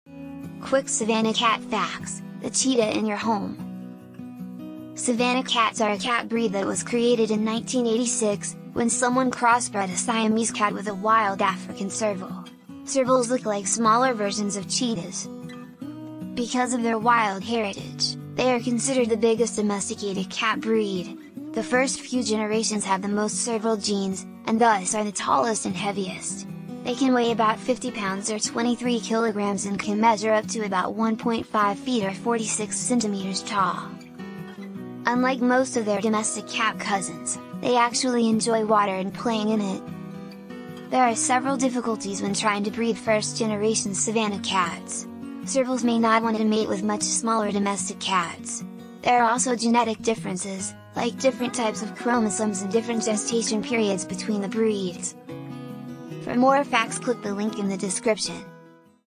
Quick Savannah Cat Facts - The Cheetah in your Home - Animal a Day
Today we learn about the large, domesticated, mini-Cheetah looking cat, the Savannah Cat.